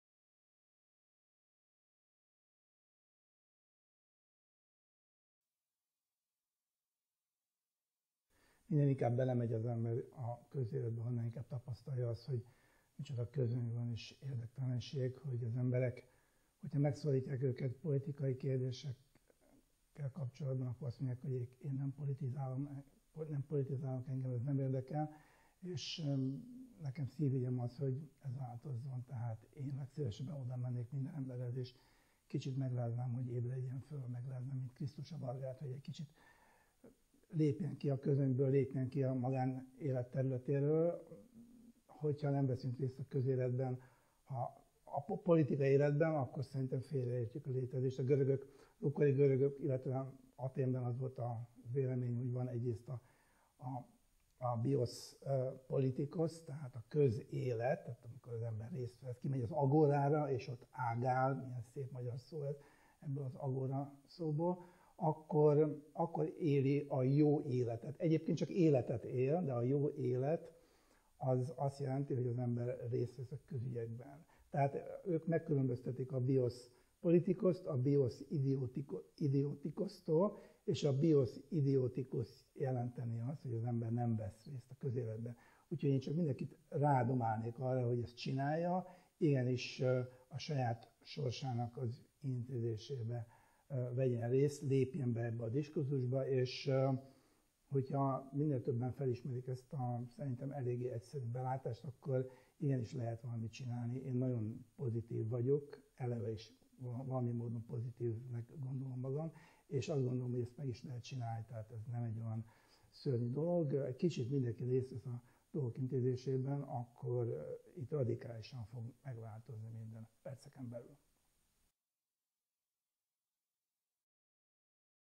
Kukorelly Endre a részvétel fontosságáról
A Polgári Platform videóüzenetében a neves író a közéleti részvétel mellett érvel. Választhatunk: "bios politicos" vagy "bios idioticos" akarunk-e lenni. Rajtunk múlik!